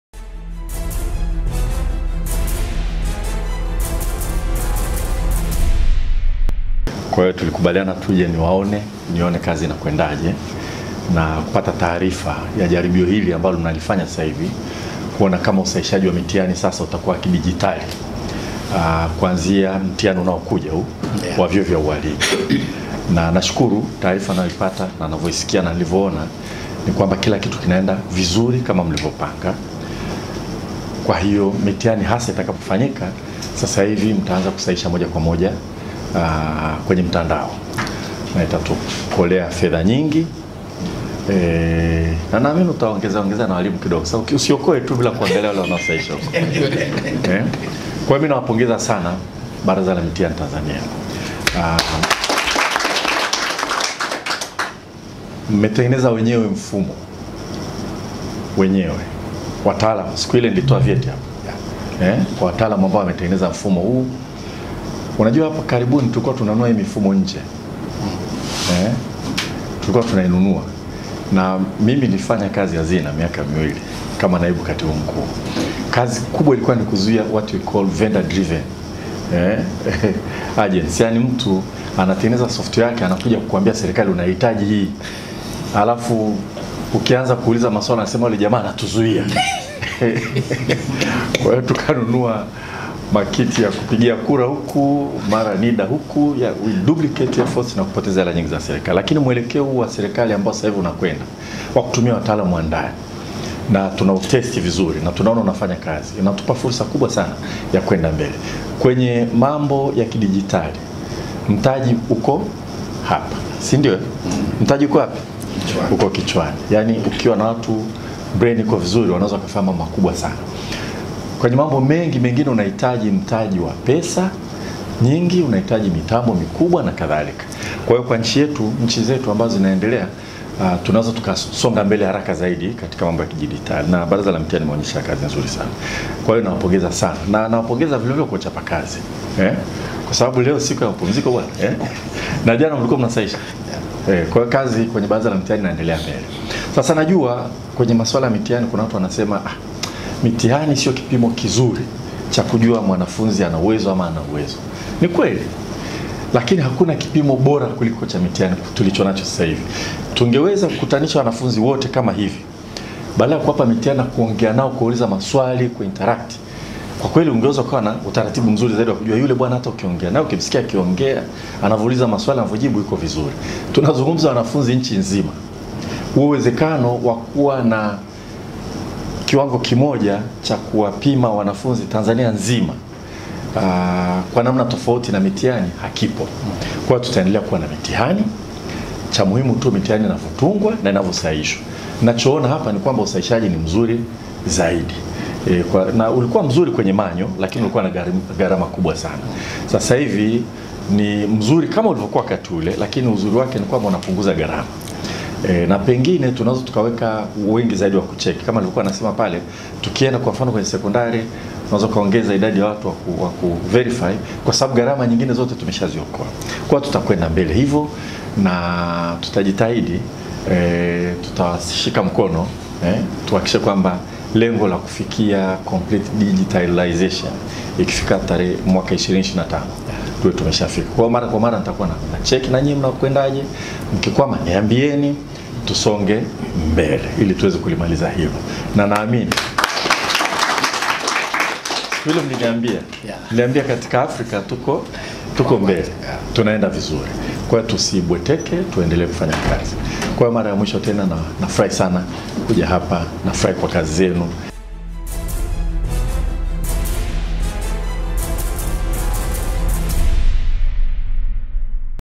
WAZIRI MKENDA Apongeza NECTA Majaribio ya Usahihishaji Mitihanii Kidigitali
Waziri wa Elimu, Sayansi na Teknolojia Mhe Prof Adolf Mkenda ameupongeza uongozi wa Baraza la Mitihani Tanzania (NECTA) kwa kusimamia kikamilifu na ubunifu mkubwa mfumo wa kisasa wa kidigitali wa usahihishaji wa mitihani nchini